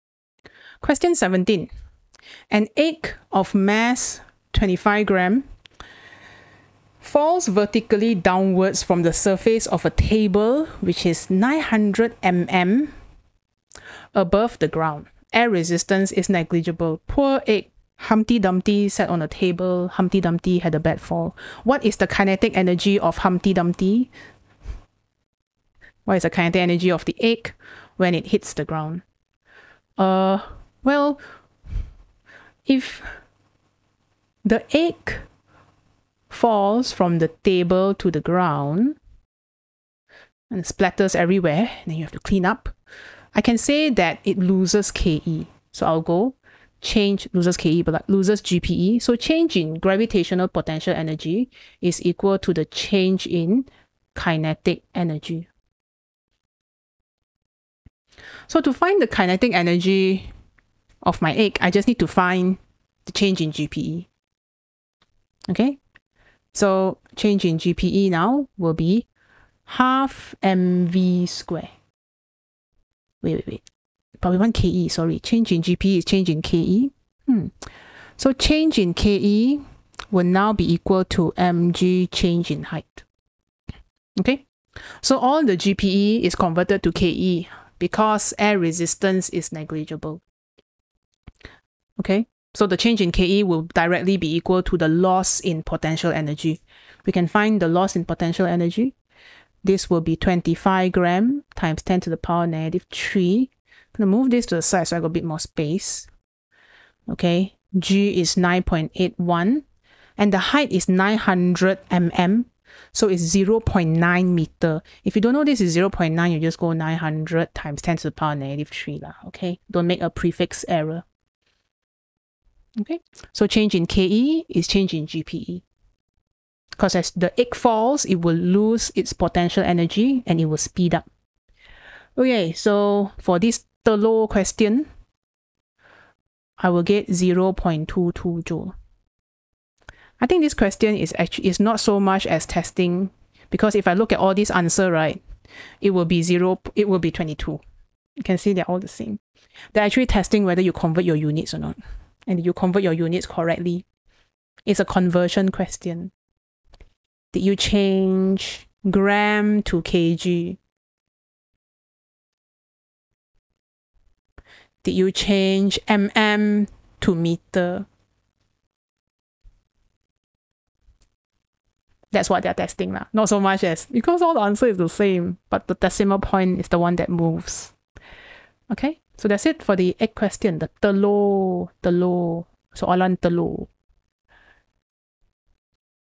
FM21 P12 Q17 Egg Drop Kinetic Energy | Feb/March 2021 | Cambridge A Level 9702 Physics
9702/12/F/M/21: An egg of mass 25g falls vertically downwards from the surface of a table which is 900mm above the ground. Air resistance is negligible. What is the kinetic energy of the egg when it hits the ground?

9702_m21_qp12